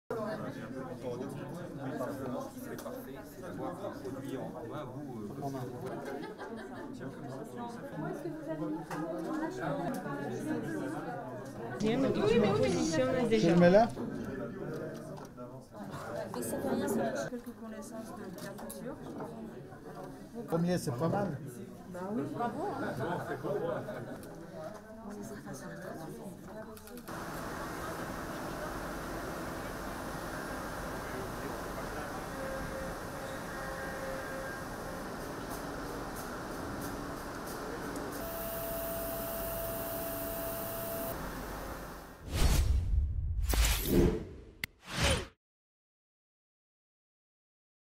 مجموعة ميشلان تفتح مصانعها جزئياً لصناعة الكمامات
فتح أحد معامل مجموعة ميشلان الفرنسية، أبوابه في مدينة كليمونت فيراند، بعد تدريب بعض العمال على تصنيع الكمامات، للمساعدة في الجهود الرامية لوقف انتشار فيروس كورونا المستجد.